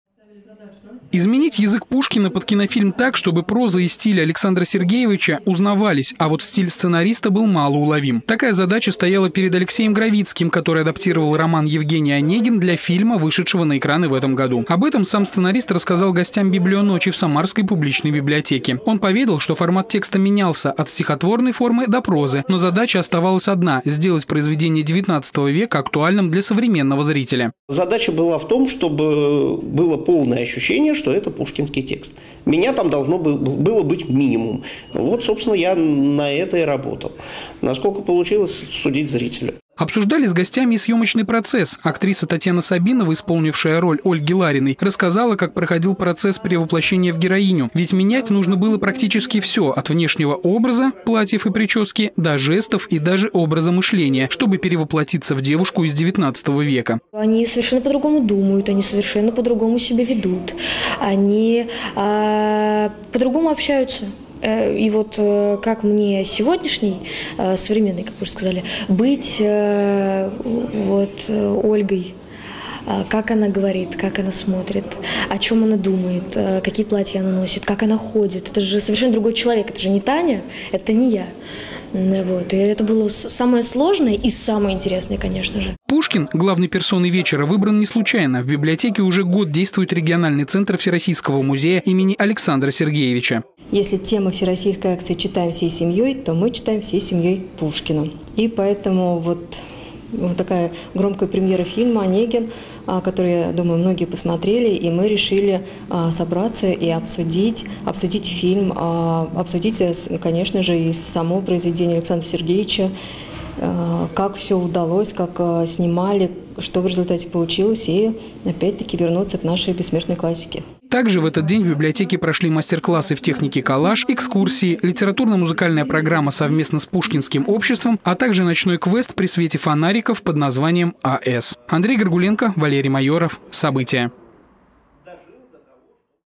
В Самаре прошла встреча со сценаристом фильма "Онегин"
День, когда можно засидеться с книжкой допоздна, и из читального зала не выгонят. Самарские библиотеки присоединились к традиционной акции "Библионочь". В этом году всероссийский литературный праздник прошёл уже в 13-й раз. Тема акции в 2024 - "Читаем всей семьёй". В самарской публичной библиотеке главным героем вечера стал   Александр Сергеевич Пушкин. Что ждало гостей вечера, расскажем в нашем сюжете.